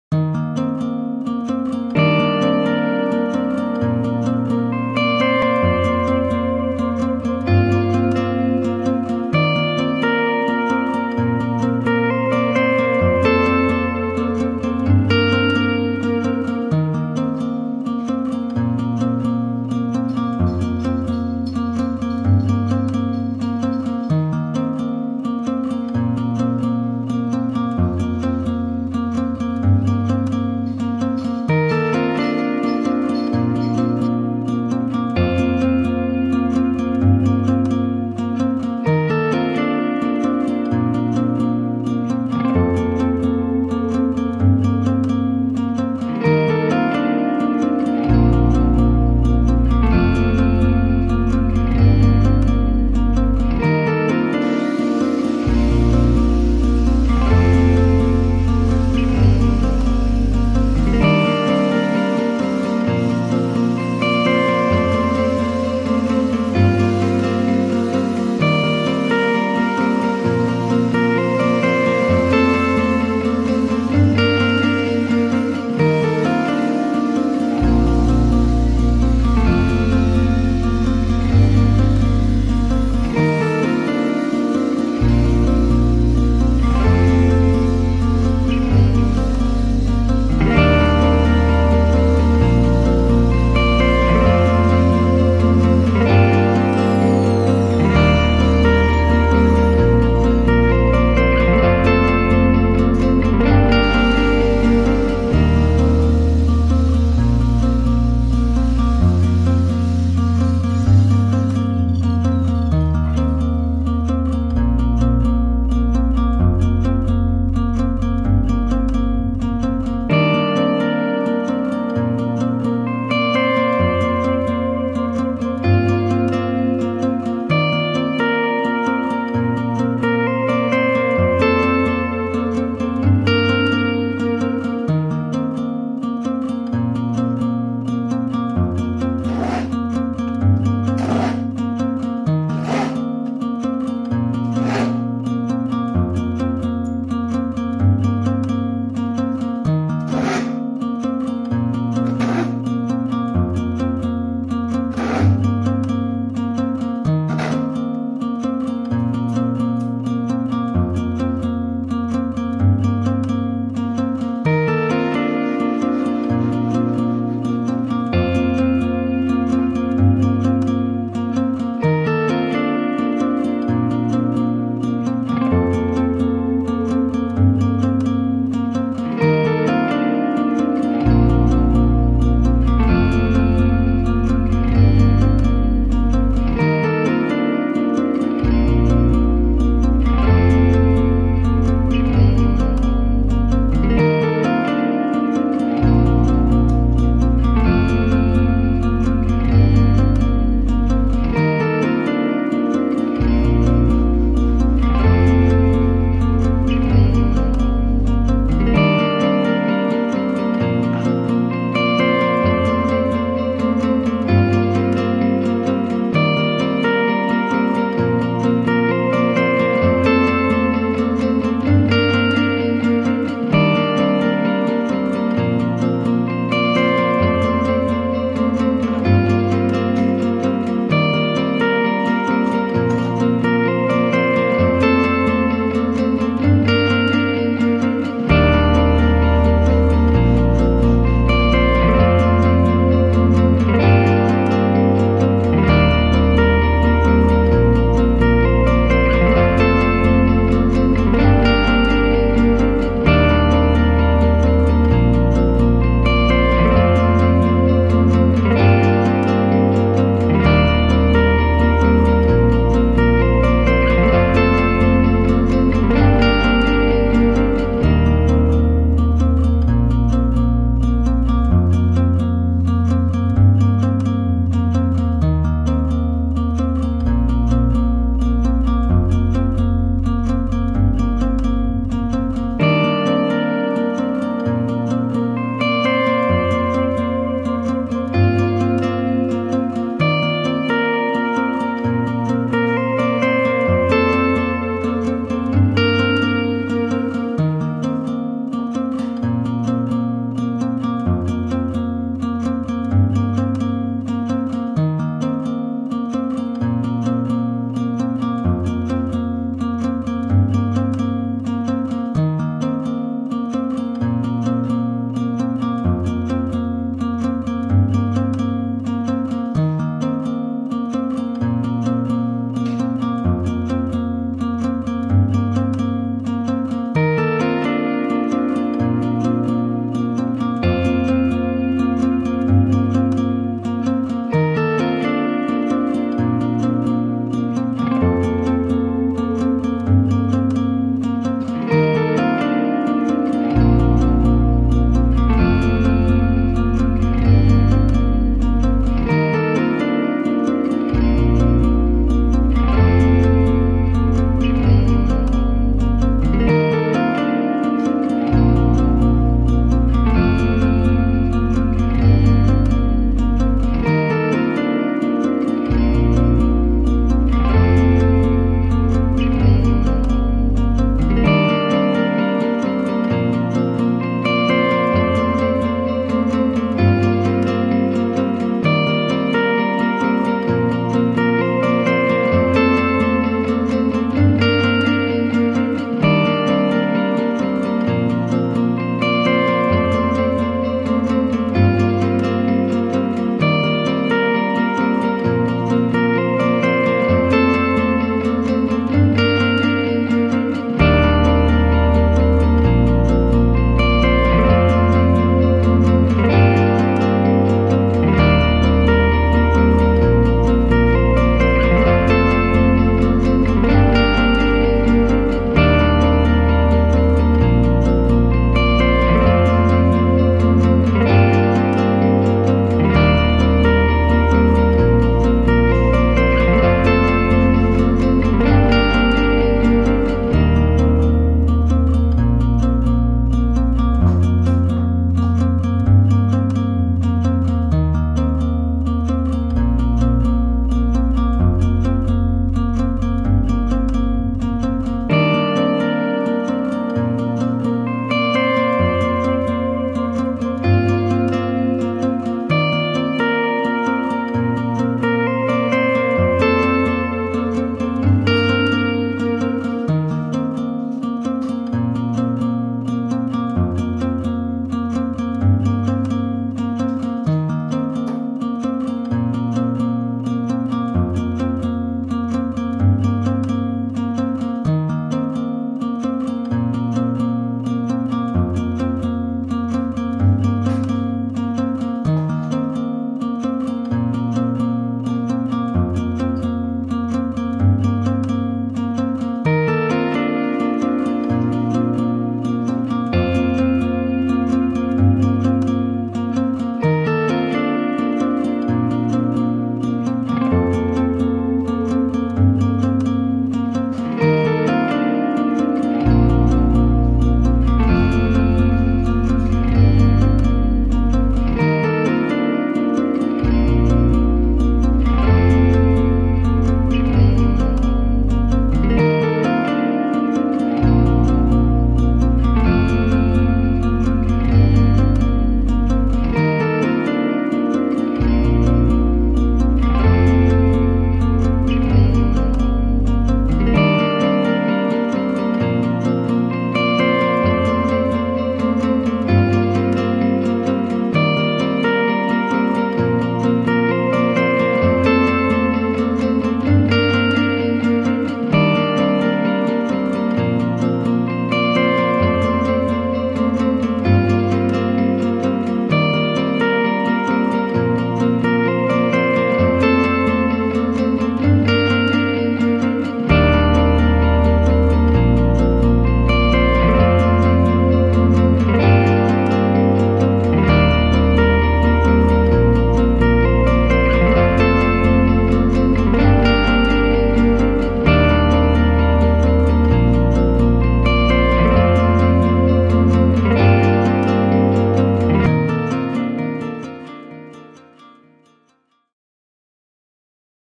Workbench 2020 - using used wood pallets
In 2020, I moved to Myanmar for work. After a few months, I realized I needed a workbench so I can work on some projects. This was is first project I did in apartment during the early pandemic lockdown/restrictions.

The woods I used are from used pallet woods. I was able to get it from the local market in downtown Yangon. I believe in reclaiming wood and that used woods can still be reused.

I'm sorry I lost some of the footage or may have accidentally deleted them.

God bless!